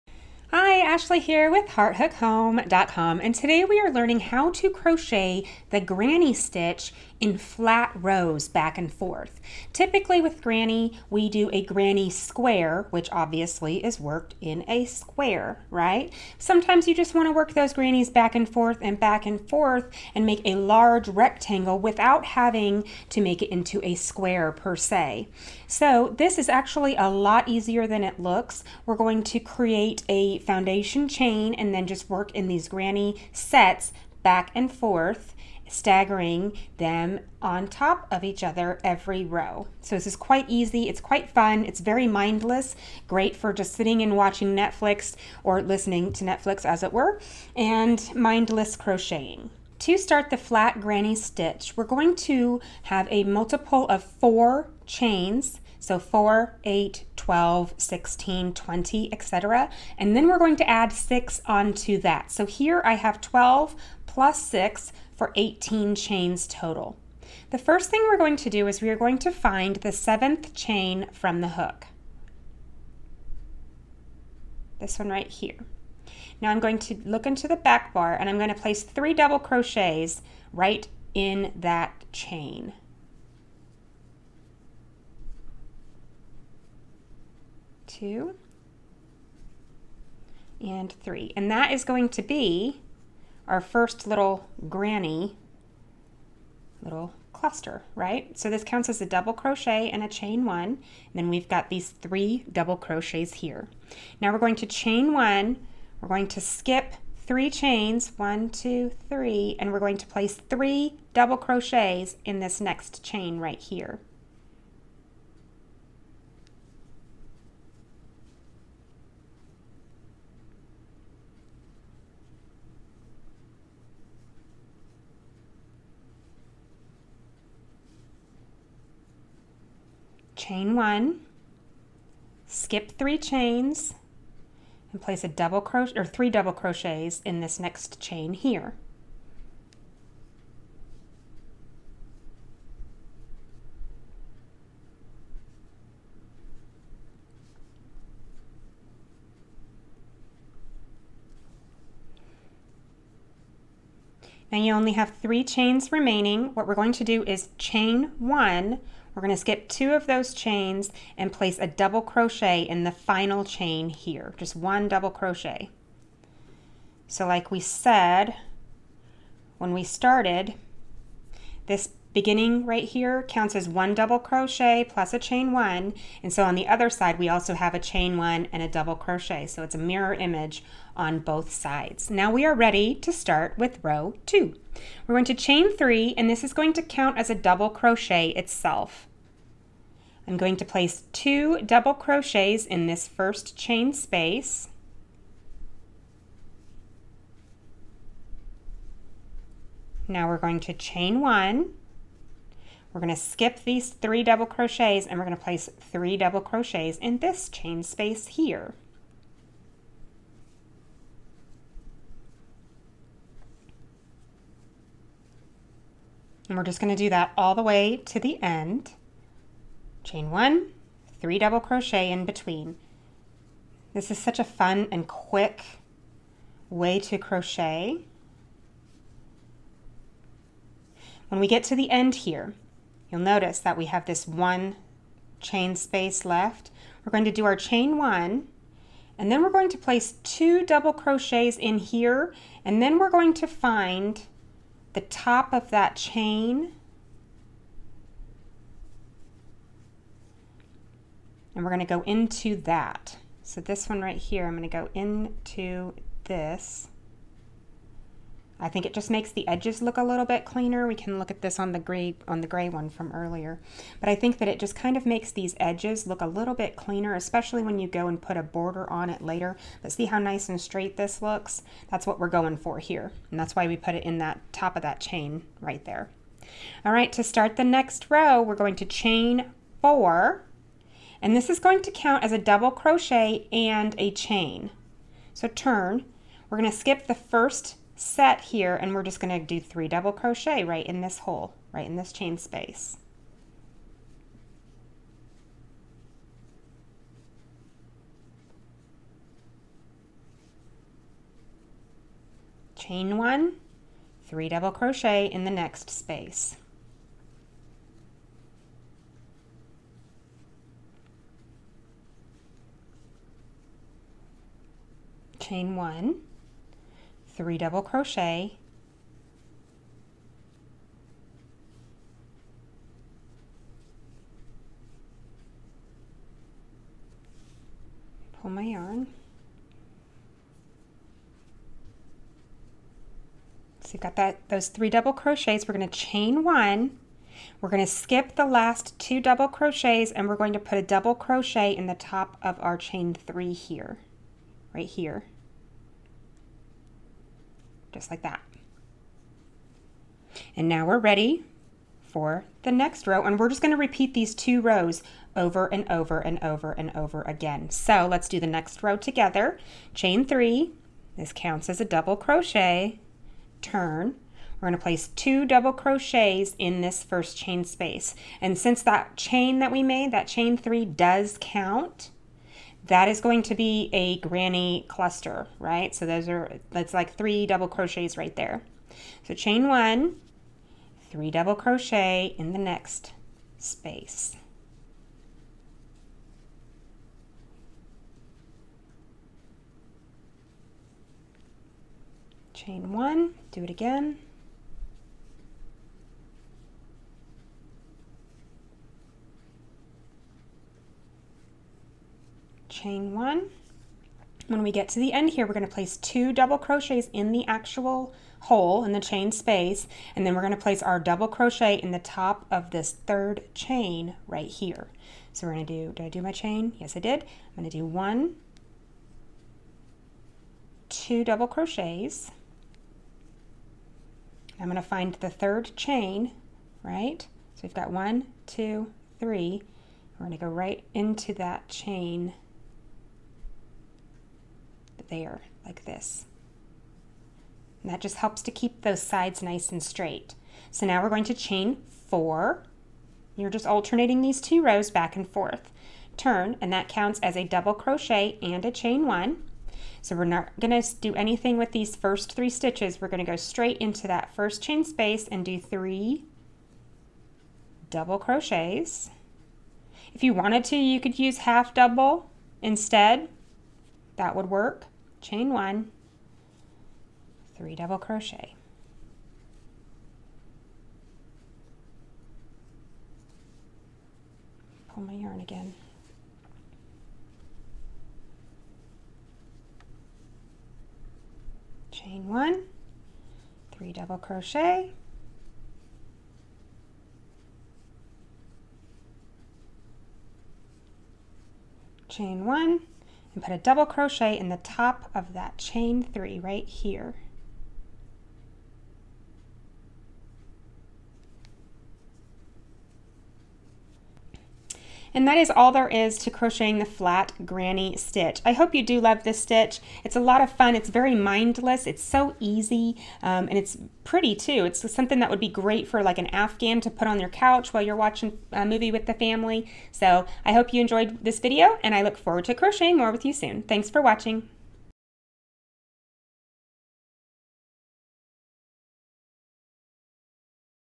Flat Granny Crochet Stitch Tutorial
Sometimes we want to just crochet the Granny Stitch in flat rows. This tutorial will help!

My favorite products: